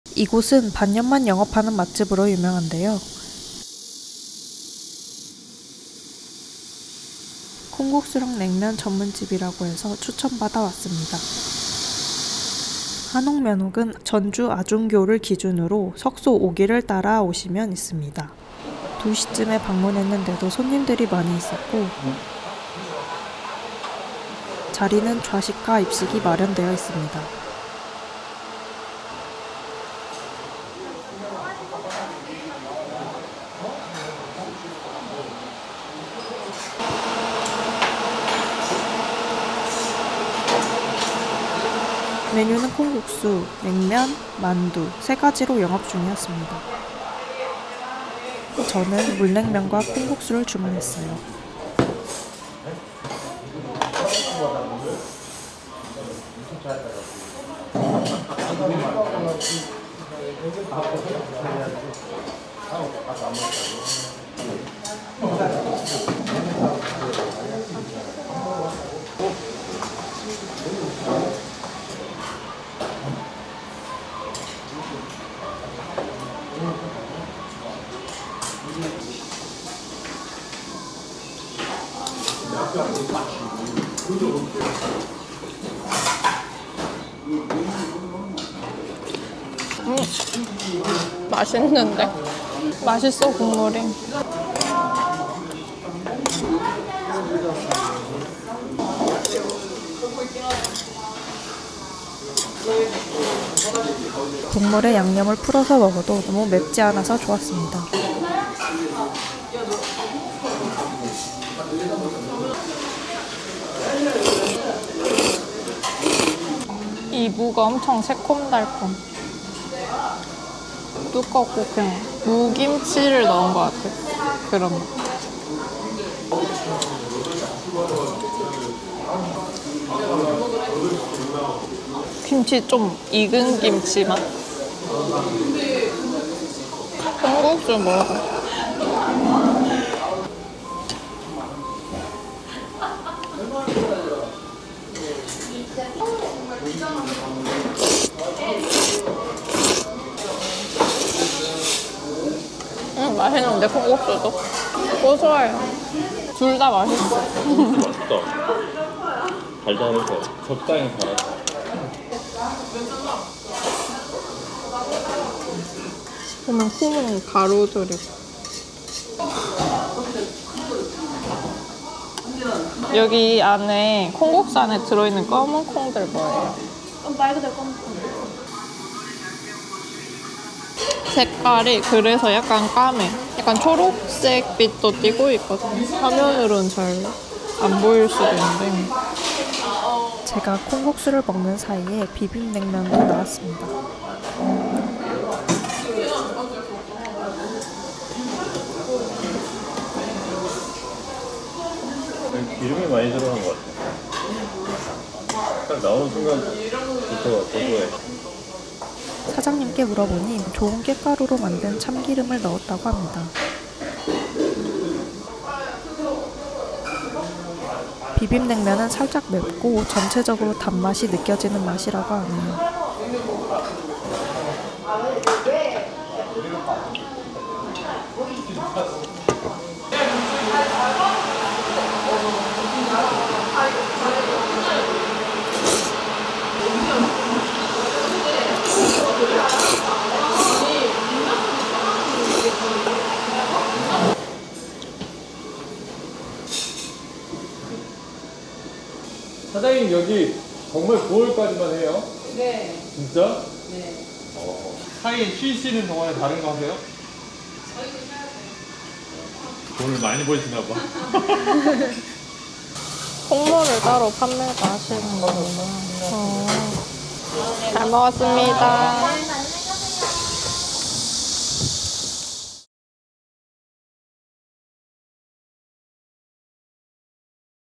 추석까지만 영업! 반년만 해도 잘 되는 맛집은 정말 맛있을까? | 전라맛집 | 재미 MADE
추석까지만 영업! 반년만 영업하고 쉰다는 사장님😎
맛집으로 입소문난 콩국수 냉면 전주 맛집을 찾아갔습니다.